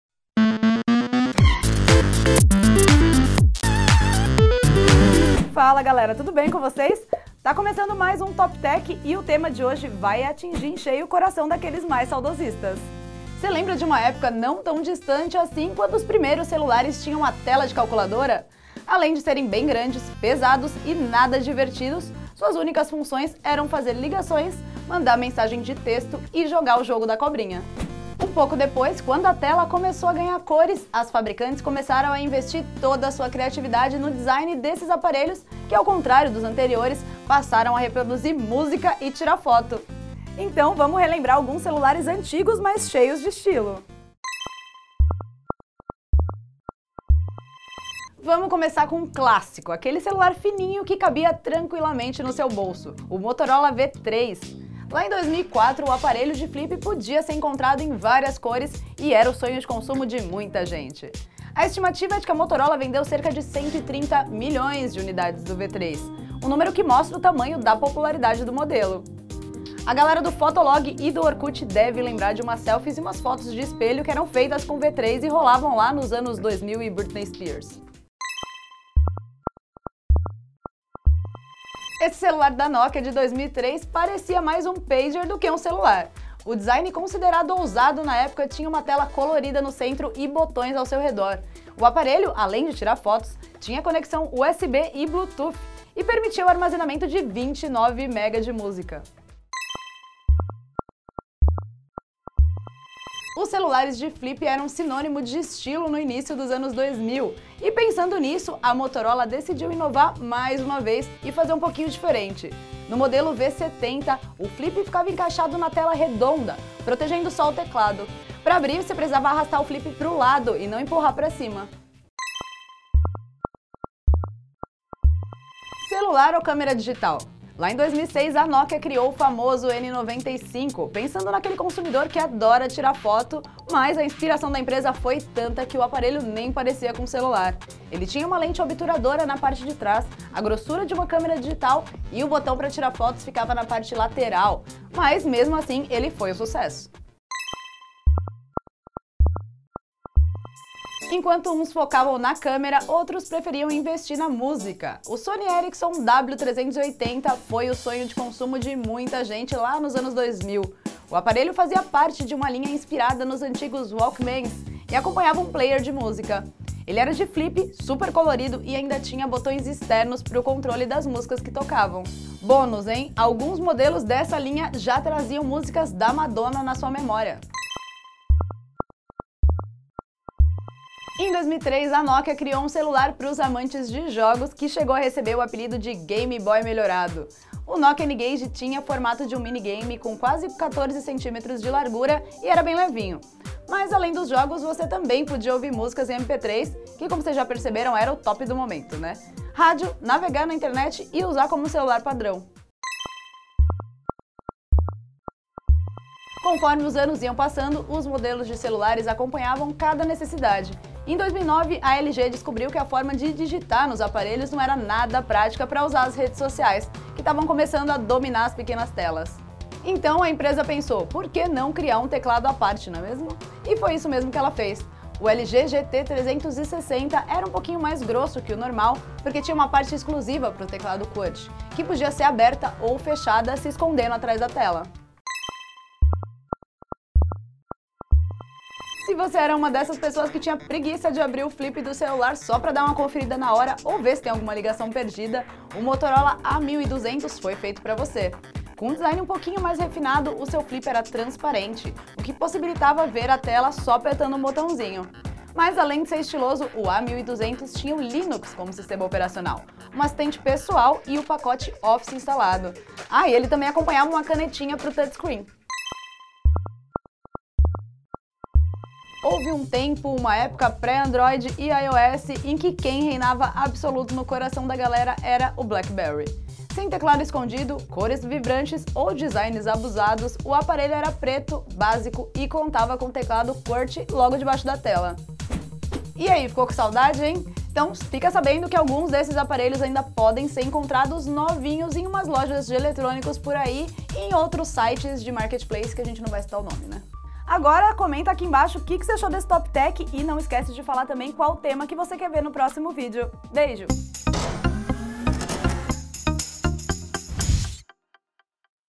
Celulares antigos com design diferentão
Você se lembra de uma época não tão distante, quando os primeiros celulares tinham "tela de calculadora"? Além de serem grandes, pesados e nada divertidos, suas únicas funções eram fazer ligações, mandar mensagens de texto e jogar o jogo da cobrinha.

SEGUNDAS:
CT News
CT Entrevista

TERÇAS:
CT News
CT Responde

QUARTAS:
CT News

QUINTAS:
CT News

SEXTAS:
CT News